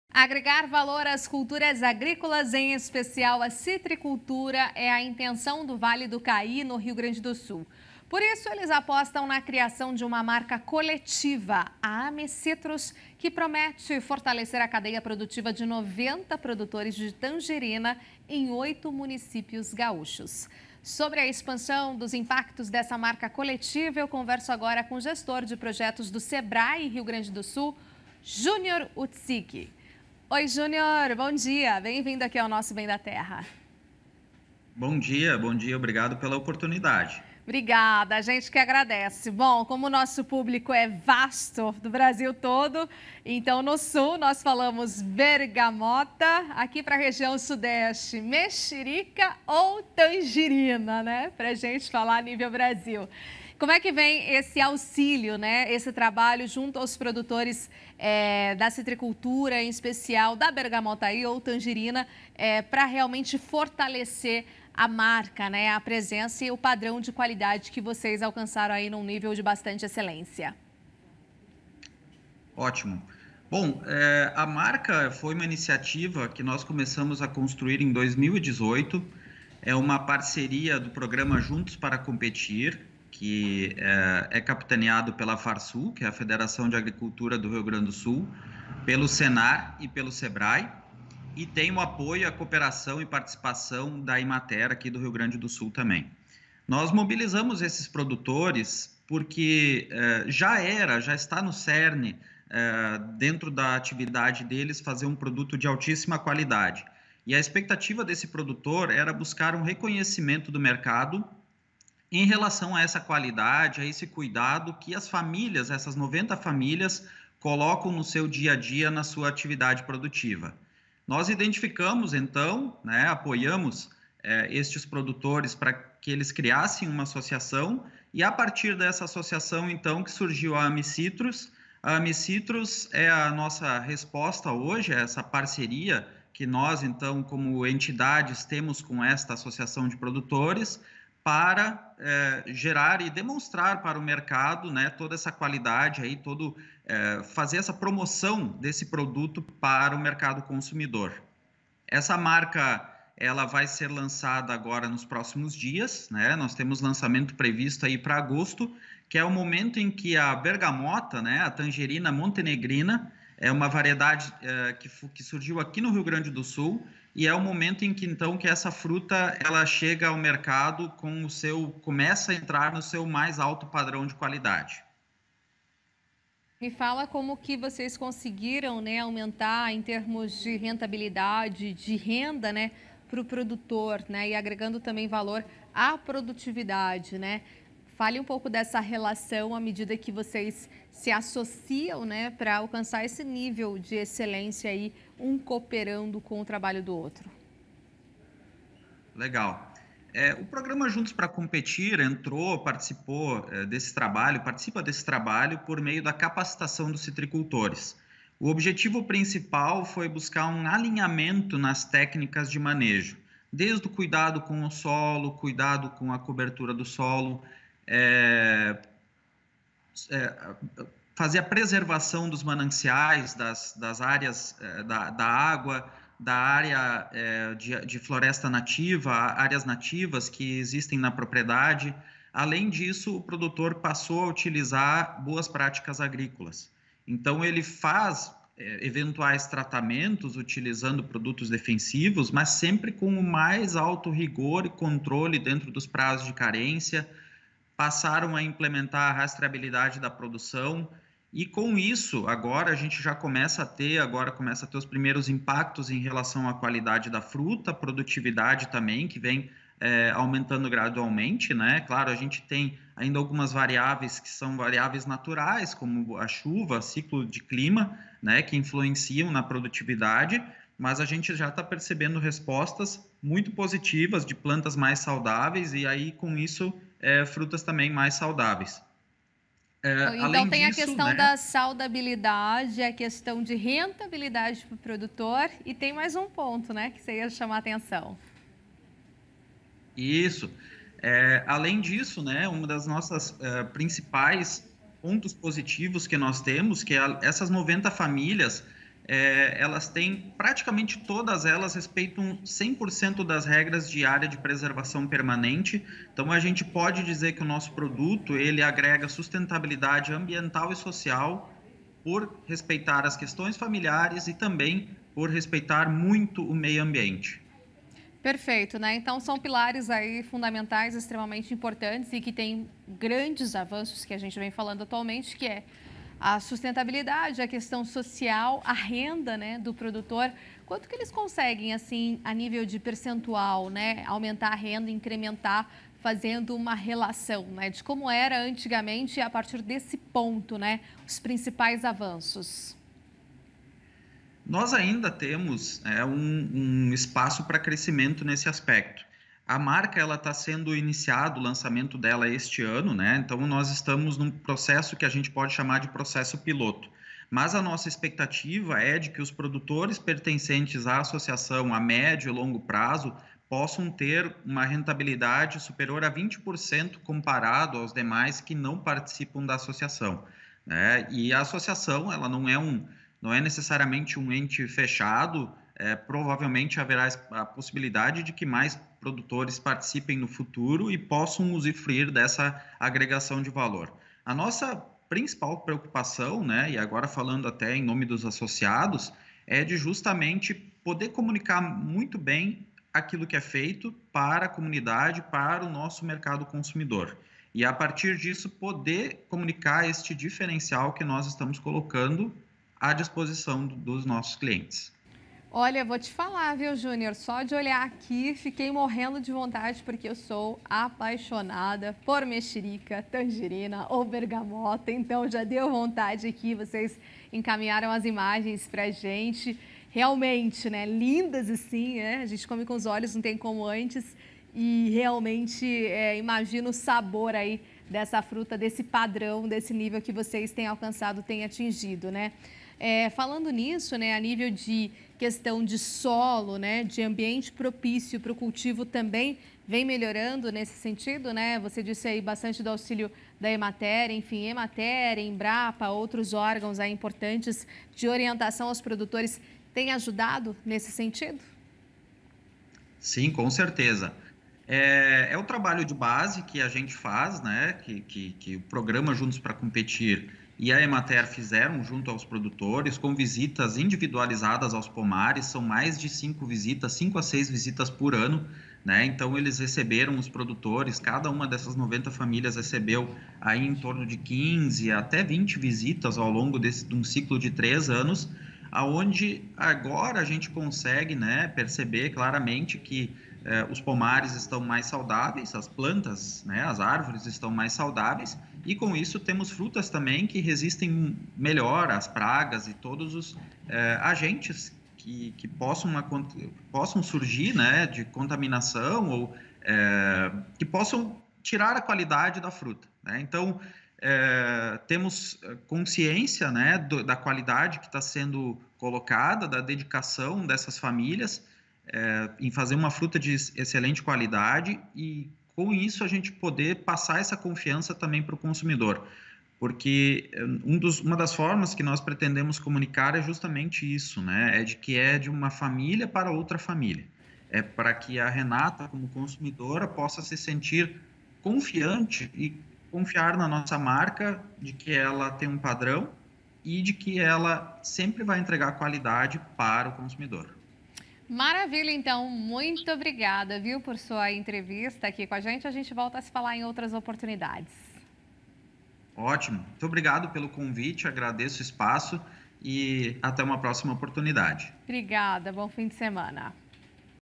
Criação de marca coletiva de tangerina deve ampliar cadeia de citricultura no Rio Grande do Sul
Agregar valor às culturas agrícolas, em especial à citricultura, é a intenção do Vale do Caí, no Rio Grande do Sul. Por isso eles apostam na criação de uma marca coletiva, a Amecitrus, que promete fortalecer a cadeia produtiva de noventa produtores de tangerina em oito municípios gaúchos.

Sobre a expansão dos impactos dessa marca coletiva, Renata Maron conversou com o gestor de projetos do Sebrae Rio Grande do Sul, Júnior Utzig.

Confira!